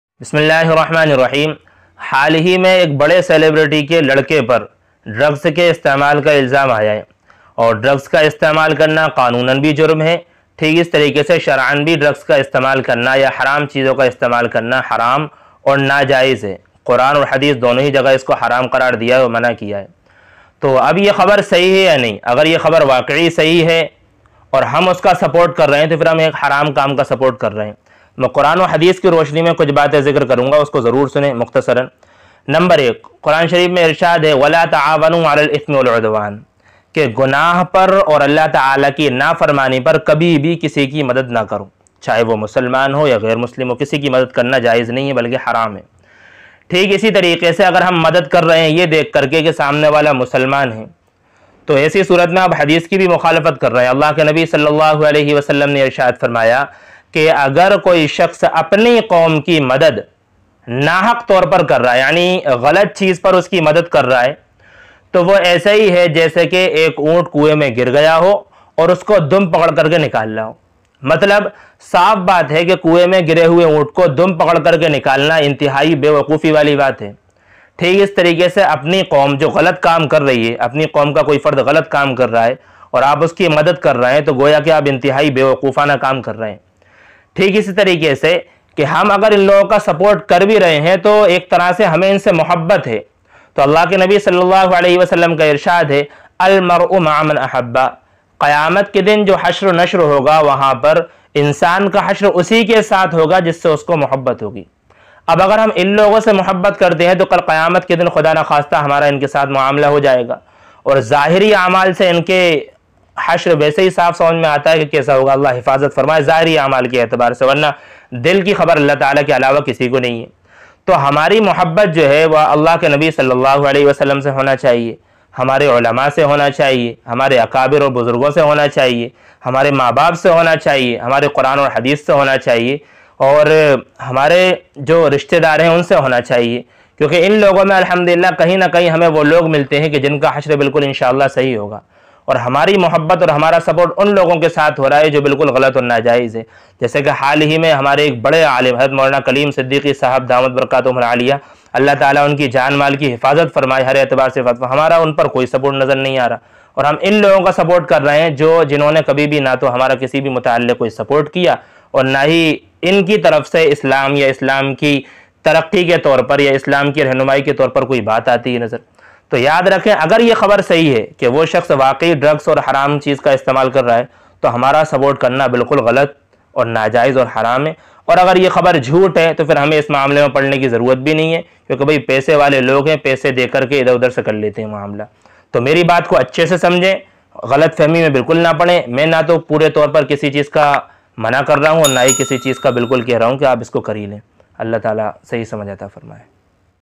Support On Shahrukh Khan Son Aryan Khan Arrested In Drugs Case | Srk's Son
Aryan khan son of shahrukh khan arrested in drugs case and some are supporting him, is this right or not we should support aryan khan or not